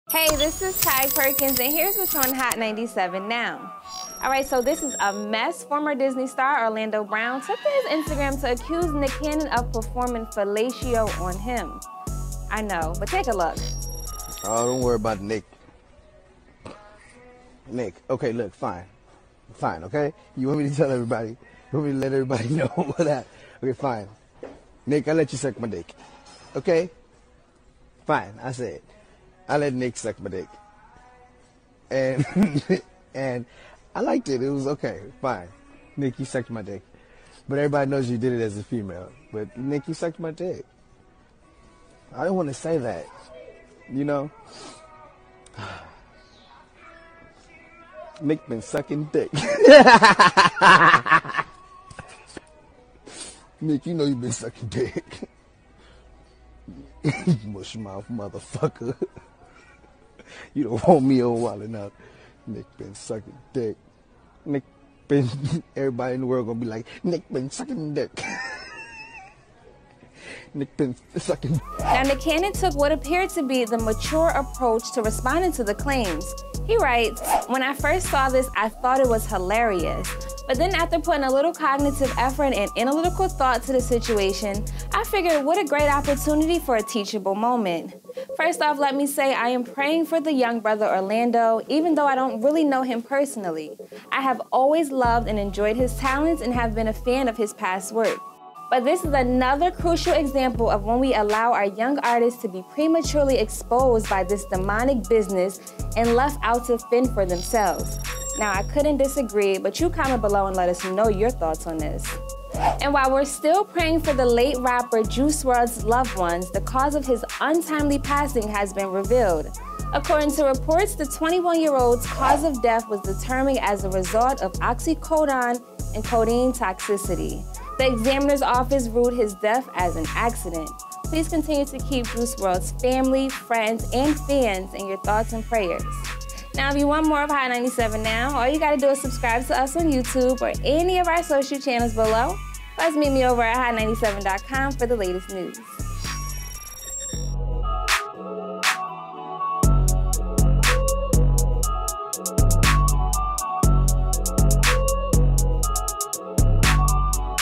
Nick Cannon Responds To Orlando Brown’s Fellatio Claims + Juice WRLD’s Cause Of Death Revealed
Nick Cannon Responds to Orlando Brown claiming he gave him fellatio, plus the cause of Juice WRLD's death has been revealed.